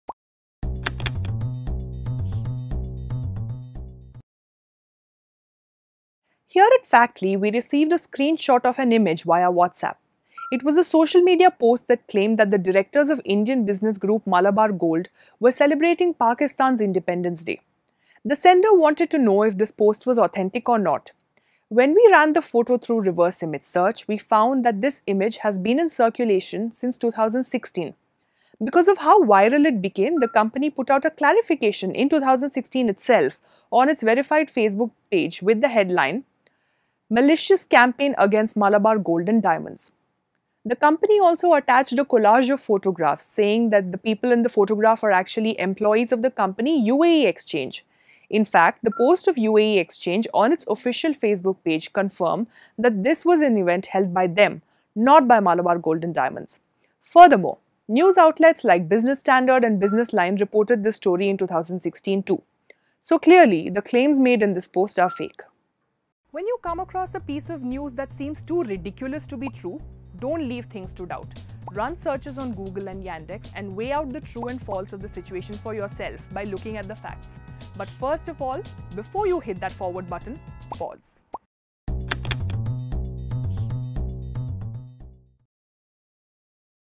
Did Malabar Gold and Diamonds celebrate Pakistan's Independence Day? | Factly
A social media post claims that the directors of the Indian company, Malabar Gold and Diamonds, celebrated Pakistan's Independence Day. We verify check the veracity of the claim in this video.